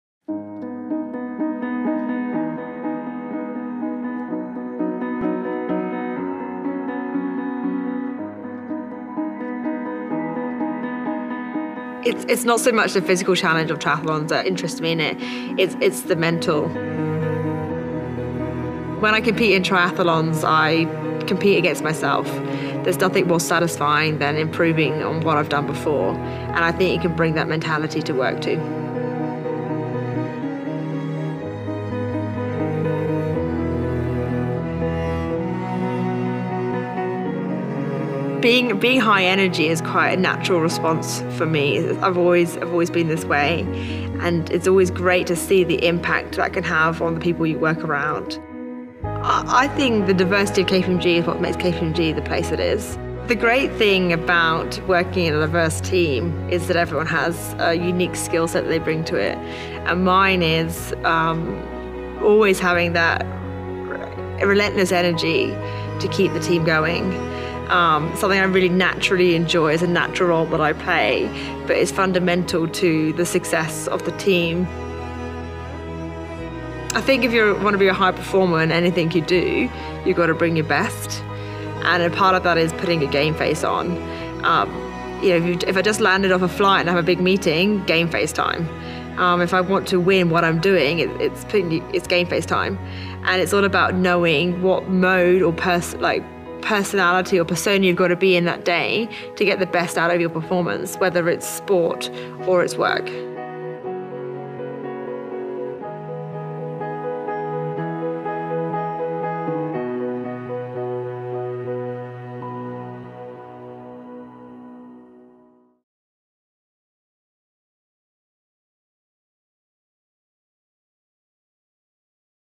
Inclusion & Diversity – Sophie Krynauw
Meet Sophie Krynauw, Senior Manager, Chief Operating Officer and part-time triathlete. Hear how Sophie’s endurance in the pool translates to the boardroom